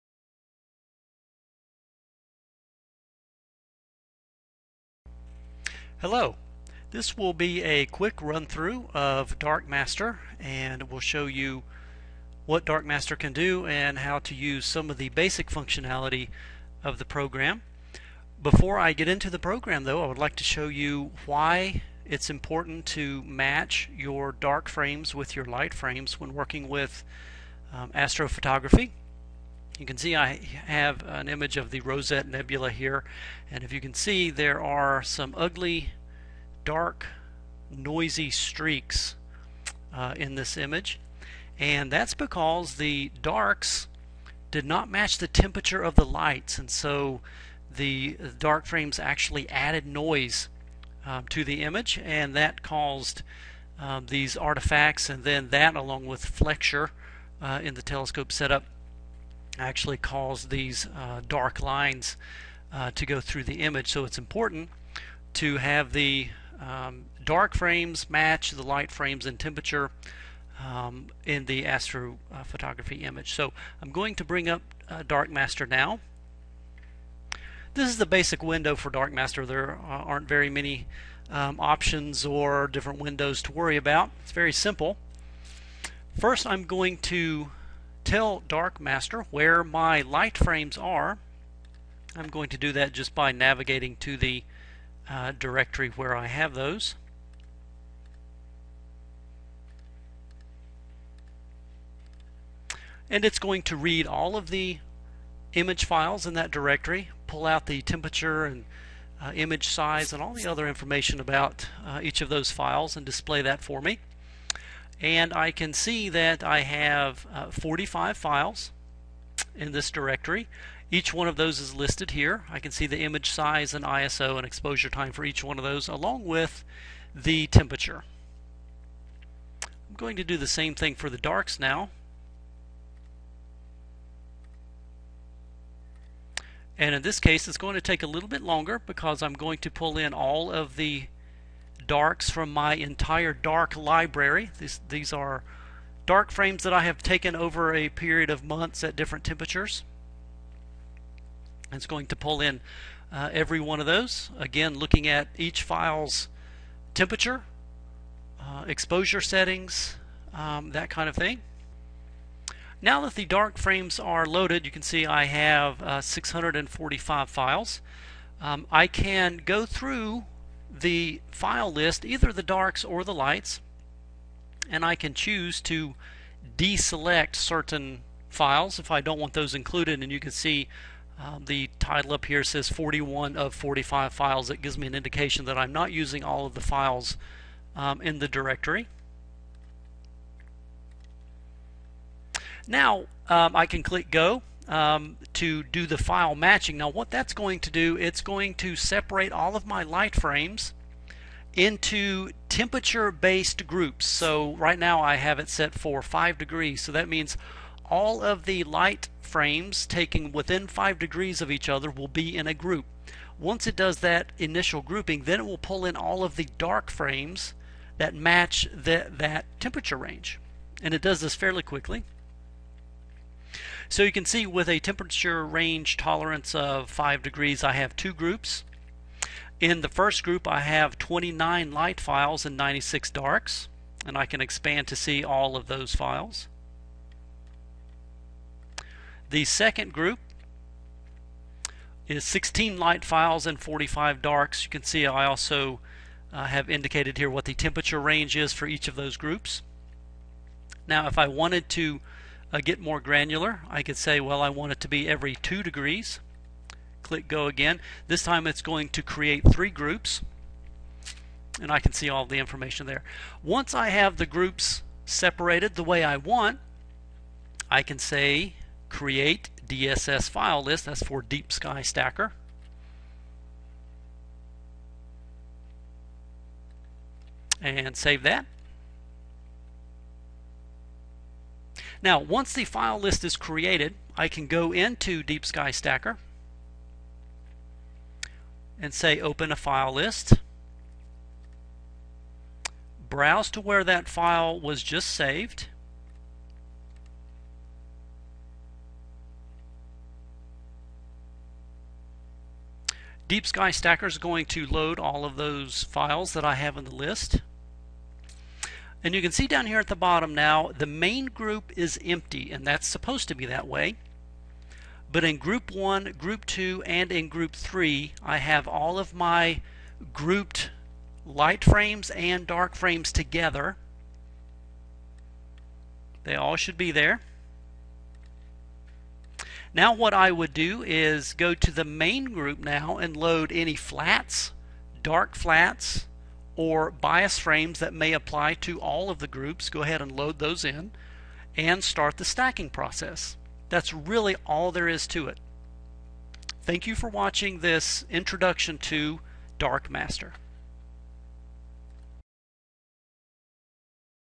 Dark Master Introduction
This video will give a brief overview of how to use Dark Master, a free application that helps organize astrophotography calibration (dark) images.